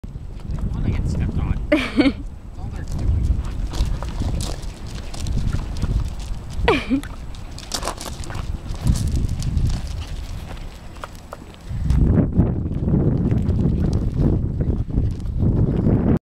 Walking with puppies
A litter of puppies makes walking a very difficult task!

8 week old Miniature Australian Shepherds